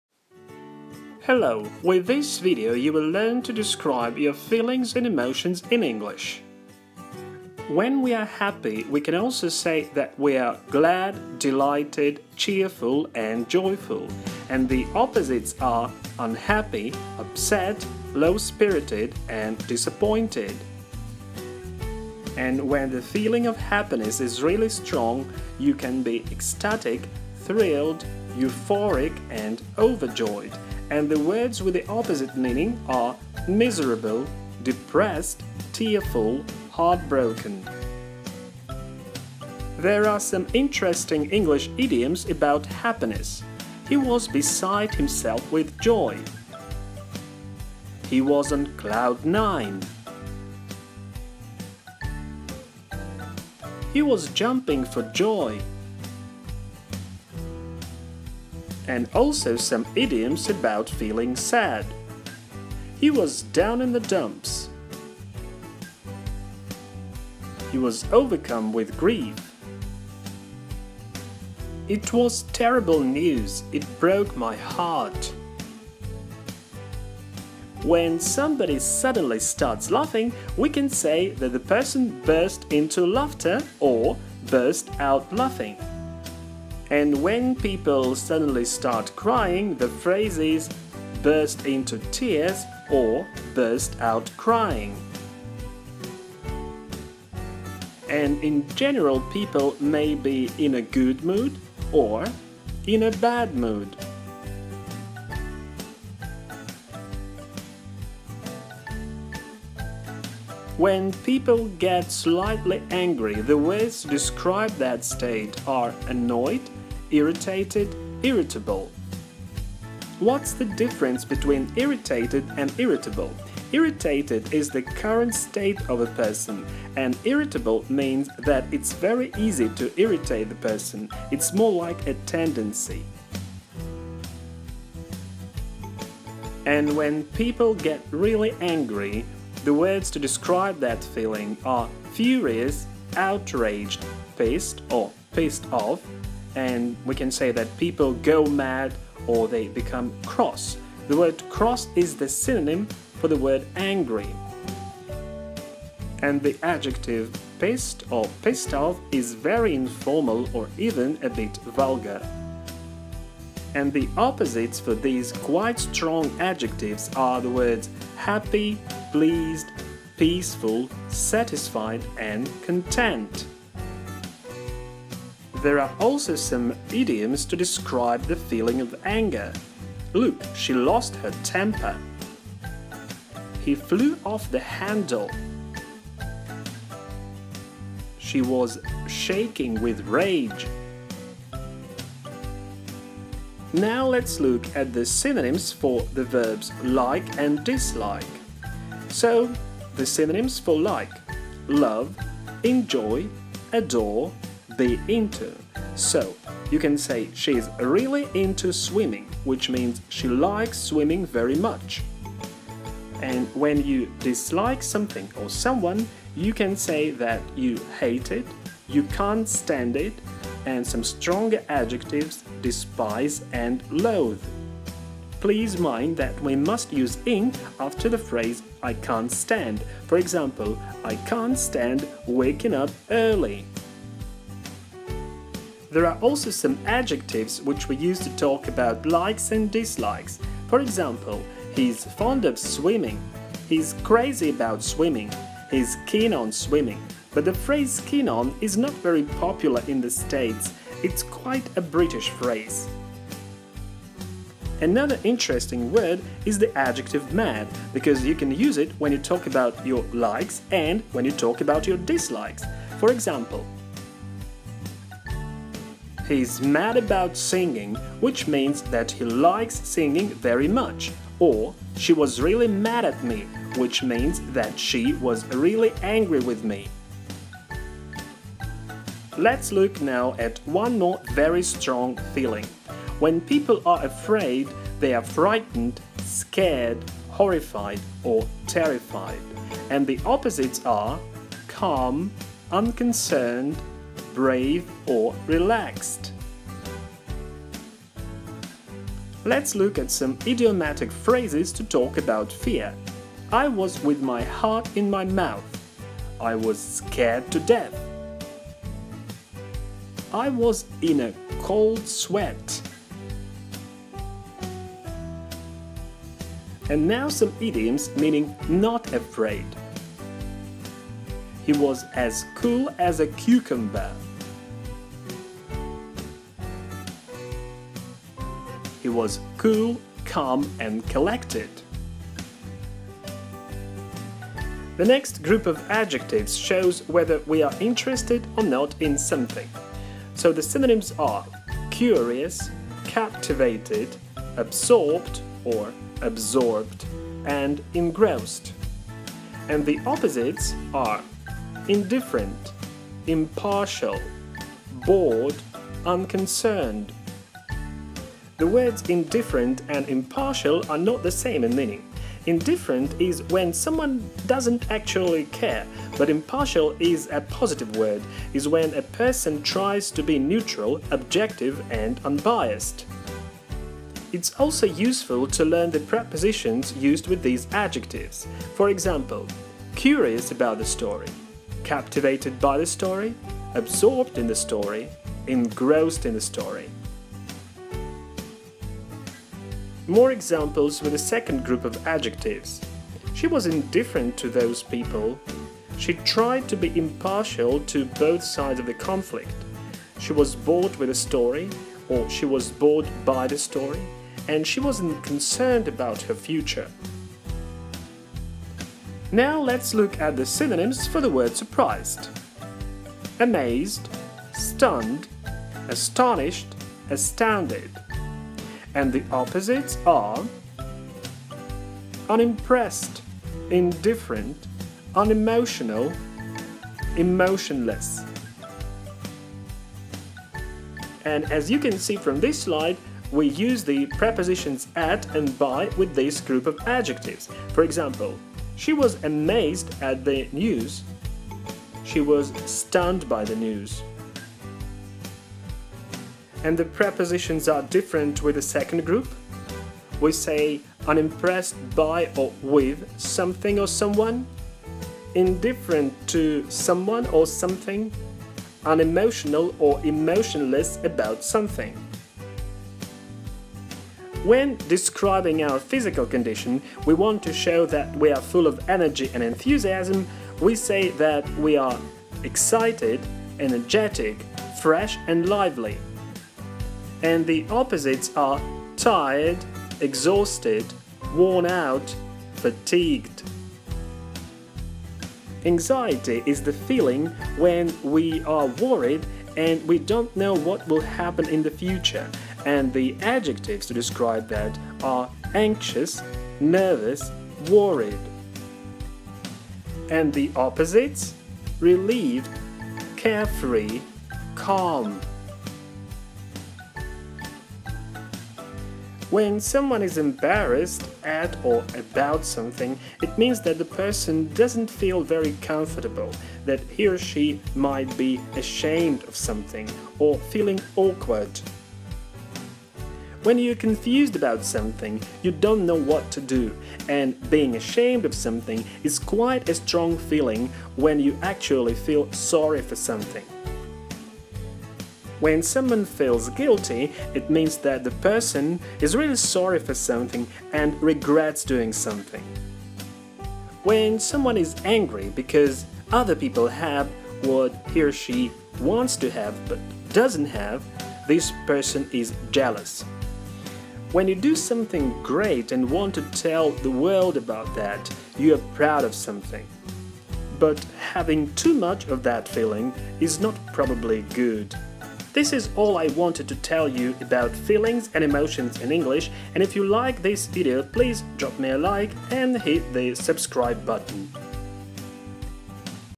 Describe Your Feelings and Emotions in English
This is the third video of the series that helps to describe people: the first one was about appearance, the second - about character and personality, and this video on feelings and emotions completes the trilogy. Here you can find not only the words and phrases on the topic, but also some related idioms and prepositional phrases.
📙Category: English vocabulary, phrasal verbs, idioms
👨‍🎓Level: Intermediate (B1+)

📄Contents:
0:07 - happy or sad
1:49 - angry or calm
3:12 - feelings related to our likes and dislikes
4:13 - double meaning of the adjective "mad"
4:39 - afraid or fearless
5:34 - interested or not
6:47 - being surprised and the words with the opposite meaning
7:43 - emotions related to our physical condition
8:05 - how to express anxiety
8:28 - other feelings: embarrassed, confused, ashamed, guilty, jealous, proud

✏️Write your examples and questions in the comments, and we will practise together!